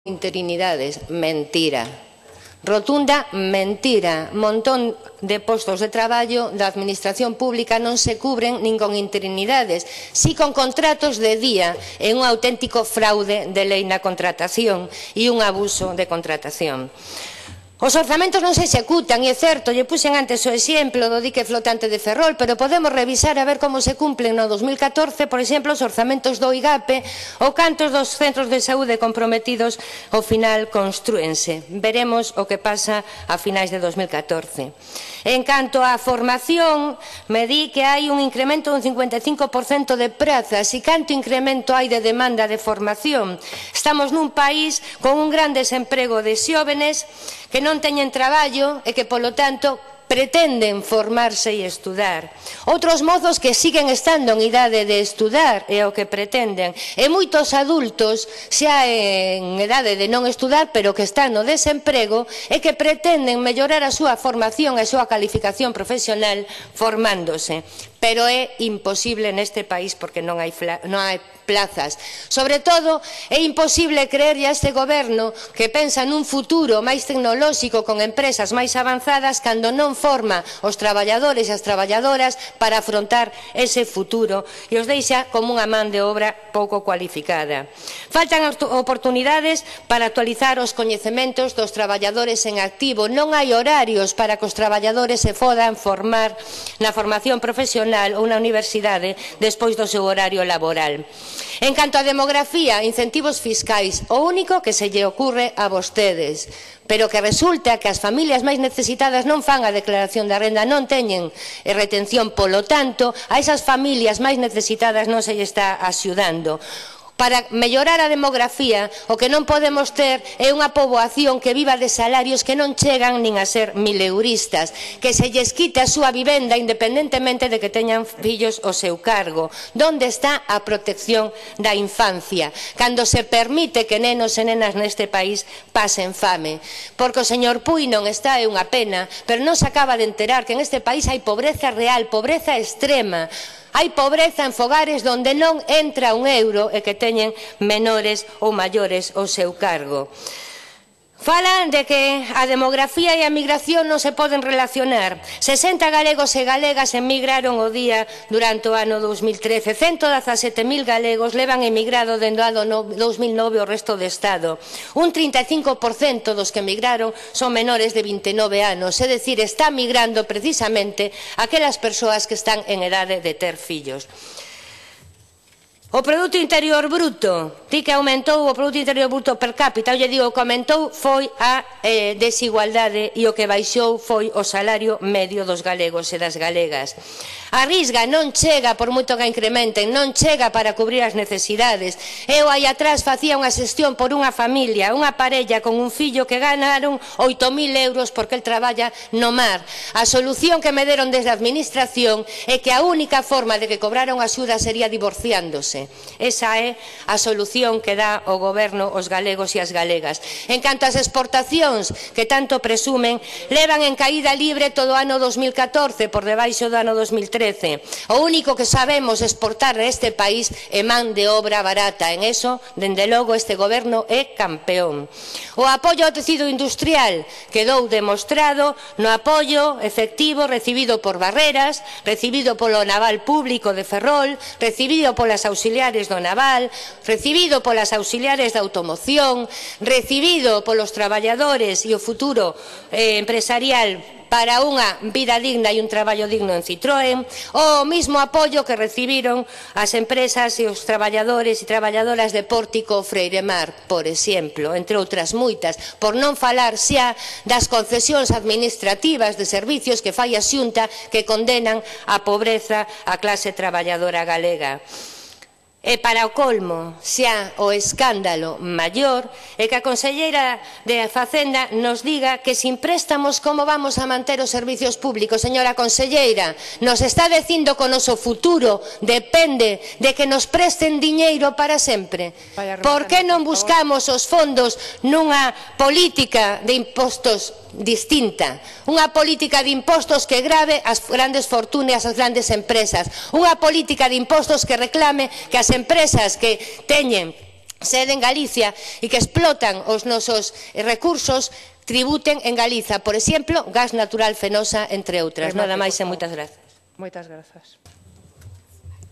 Os Orzamentos 2015 son un cúmulo de mentiras
Cómpre unha política de impostos distinta, que grave a quen máis ten e que obrigue ás empresas que explotan os nosos recursos a tributar en Galiza.